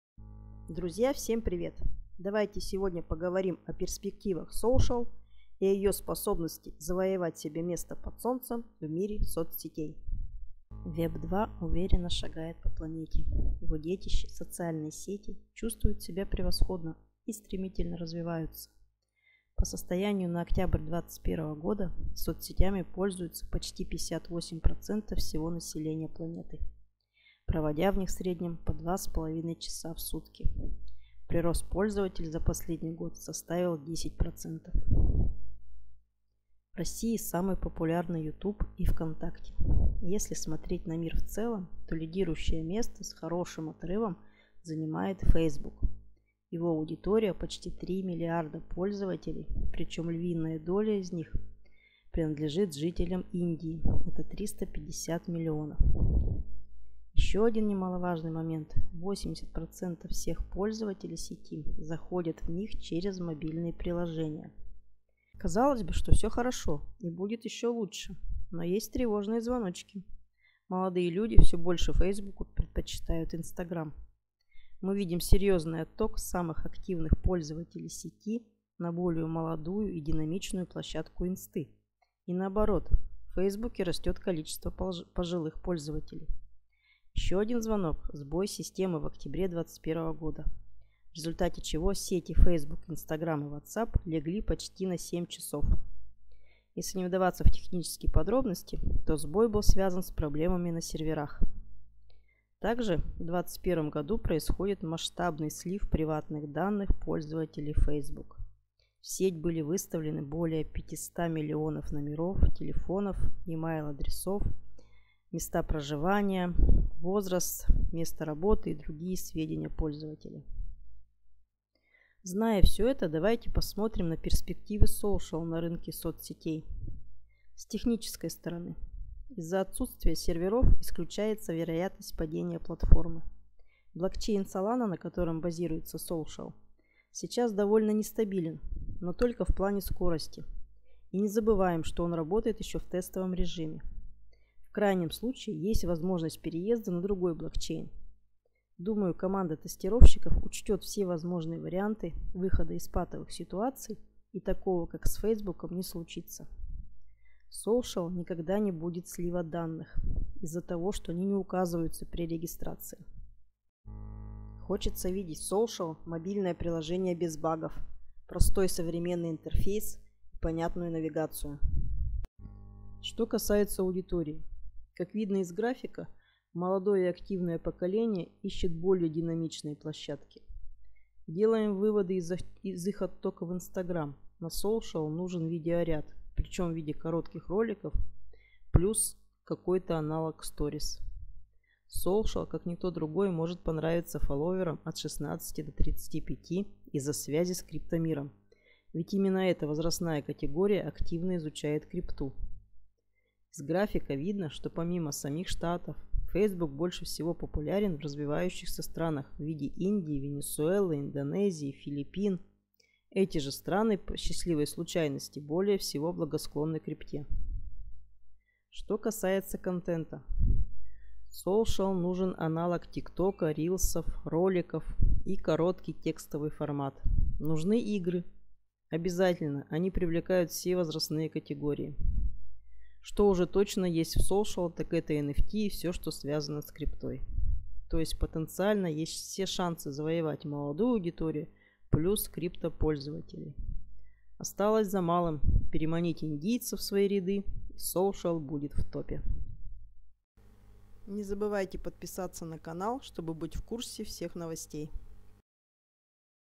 Сможет ли Solcial победить Facebook
Проблемы с приватностью в Facebook и как Solcial может закрыть эту боль пользователей...